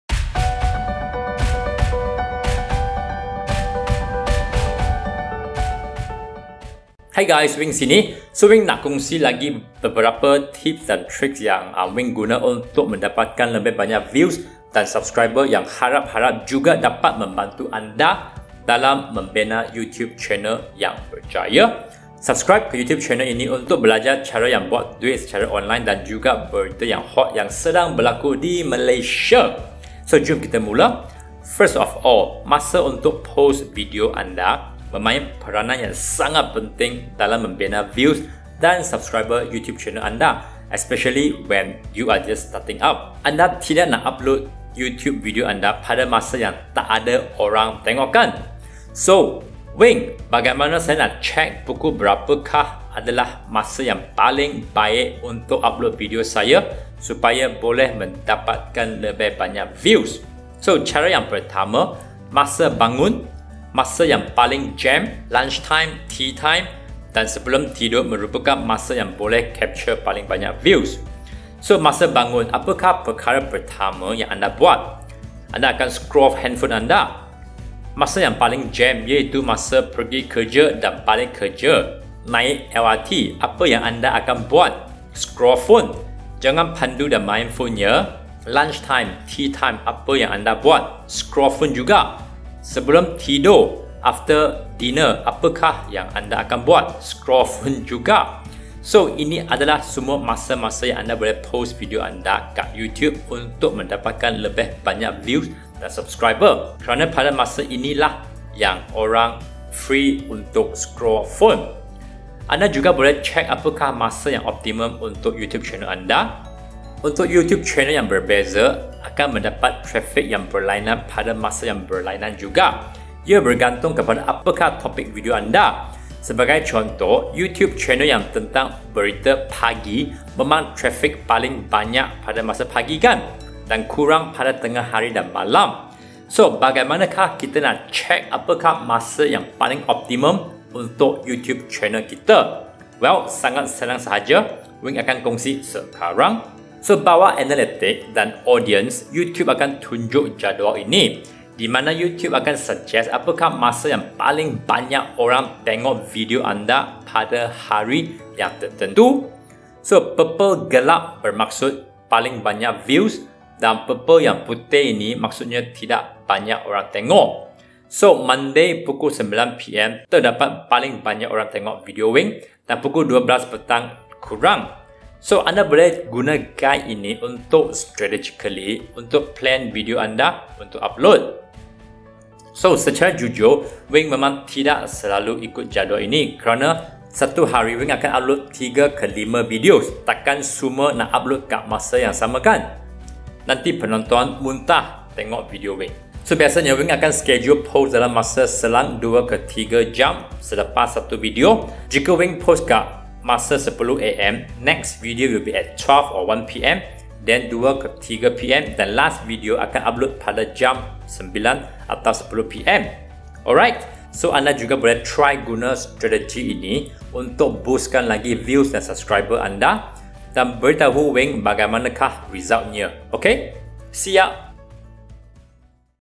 Cara BOOST VIEW DAN SUBSCRIBERS CHANNEL YOUTUBE ANDA
Youtube Ninja 1,000,000 Views Video Programme
Hi guys Weng sini.
So , Weng nak kongsi lagi beberapa tips dan trick yang Weng guna untuk mendapatkan lebih banyak views dan subscriber yang harap harap juga dapat membantu anda dalam membina youtube channel yang Berjaya.
Jom kita mula .
First of all , masa untuk post video anda memain peranan yang penting dalam membina views dan subscribers youtube channel anda.
Anda tidak nak upload youtube video anda pada masa yang tak ada orang tengok kan?
So , weng bagaimana saya nak check pukul berapa adalah masa yang paling baik yang upload video saya supaya boleh dapatkan lebih baik views ?
Cara pertama ,masa bangun, masa yang paling jam , lunch time ,tea time dan sebelum tidur merupakan masa yang boleh capture paling banyak views.
Masa bangun , apakah perkara pertama yang anda buat? Scroll phone .
Masa paling jam , iaitu masa pergi kerja , naik lrt ,apa yang anda buat scroll phone.jangan pandu dan main phone ya.
Lunch time , tea time , apa anda buat , scroll phone
Sebelum tidur apa anda buat ? scroll phone jugak.
So , ini adalah masa masa yang anda post video anda kat youtube untuk mendapatkan lebih banyak views dan subscribers.
Anda juga boleh check apakah masa yang optimum untuk youtube channel anda.
Youtube channel yang tidak sama , akan mendapat traffic yang berlainan pada masa yang belainan.
Ia bergantung kepada apakah topic video anda. Sebagai contoh youtube channel yang tentang berita pagi , memang traffic paling banyak masa pagi dan kurang pada tengah hari dan malam kan?
So , bagaimana anda nak check , apakah masa yang paling optimum untuk youtube channel anda?
Sangat senang .
Bawah analytics dan audience youtube akan tunjuk jadual ini , di mana youtube akan suggest apakah masa yang paling banyak orang tengok video anda pada hari yang tertentu.
Purple gelap maksud yang views paling tinggi dan purple putih maksudnya tidak banyak orang tengok .
So Monday pukul 9pm terdapat paling banyak orang tengok video Weng , dan pukul 12 petang kurang .
Anda boleh guna guide ini , untuk strategically plan video anda untuk upload.
Secara jujur , tidak selalu ikut jadual ini , kerana , saya hari Weng akan upload 3-5 videos .tak kan semua nak upload kat masa yang sama kat.
Nanti penonton muntah tengok video Weng .
Weng akan schedule post dalam masa yang selang 2-3 jam selepas satu video.
Jika Weng post pada masa 10 am , next video will be 12-1pm then 2-3pm last video akan upload pada masa 9-10pm .
Ok , anda boleh juga try guna strategy ini untuk  boostkan views dan subscribers anda.dan beritahu Weng bagaimanakah resultnya ya.
See ya